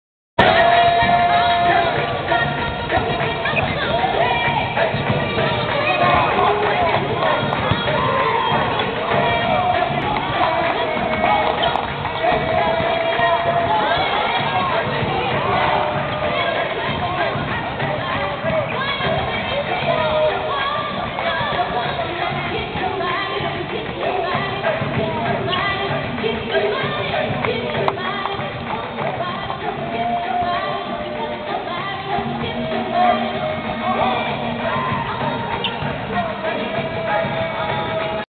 Tennessee Vols bball before SC game
Tn Vols warm up before SC game 2008 in Columbia